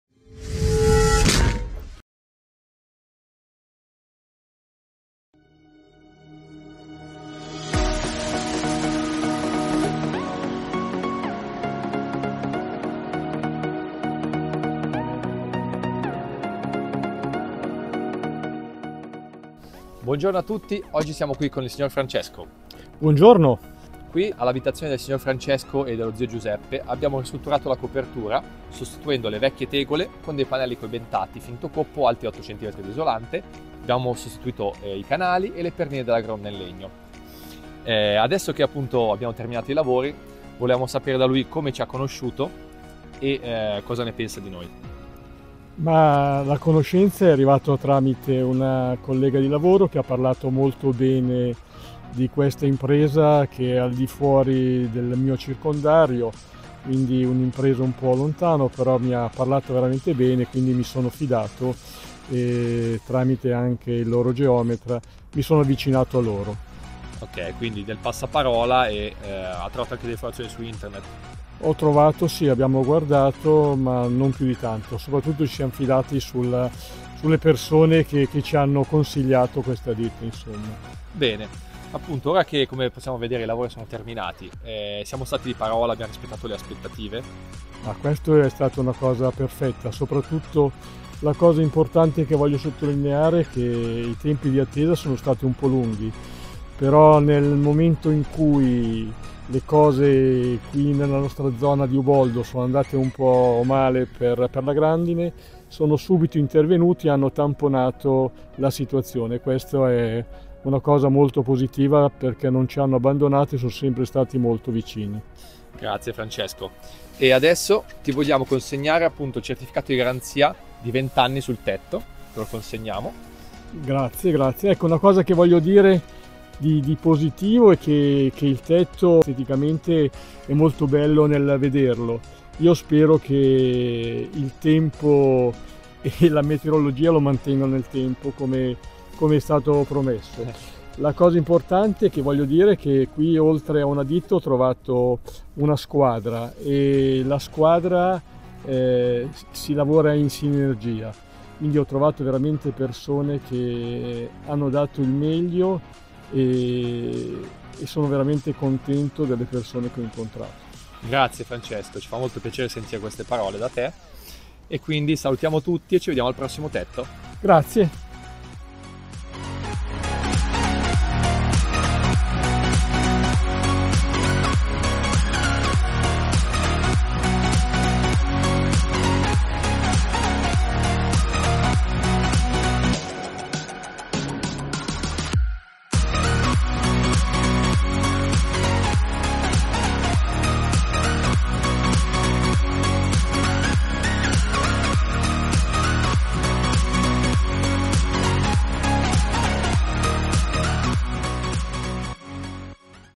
Testimonianza Cliente | Rifacimento Tetto con Pannelli Coibentati | Reginato - Tetti Italiani
Scopri la testimonianza del signor Francesco dopo il rifacimento del tetto di casa sua a Uboldo. Abbiamo sostituito le vecchie tegole con pannelli coibentati effetto coppo e garantito un tetto esteticamente impeccabile e isolato per 20 anni. Francesco ci racconta come ci ha conosciuto, l’esperienza positiva con il nostro team e l’importanza del passaparola.

✅ Un tetto garantito 20 anni
✅ Lavoro svolto con precisione e professionalità
✅ Squadra affidabile e attenta alle esigenze del cliente